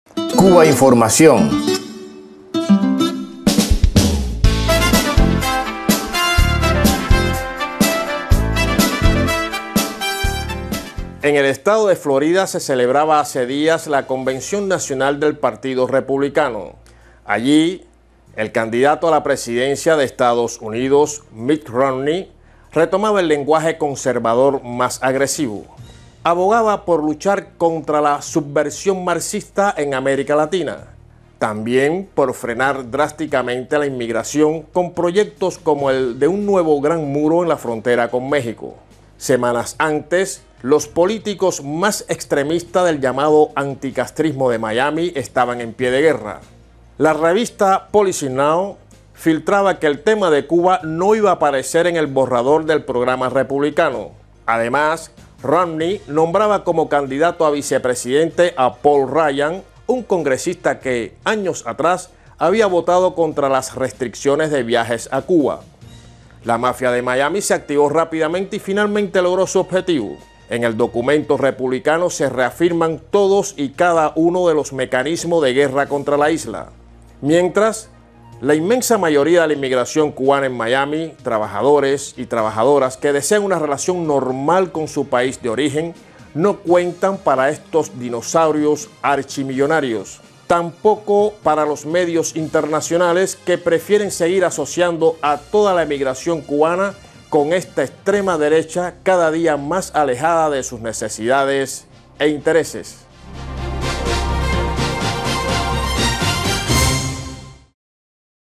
Convención republicana en Florida: dinosaurios contra Cuba
Basado en un texto de Iroel Sánchez, en el Blog "La pupila insomne" (Cuba)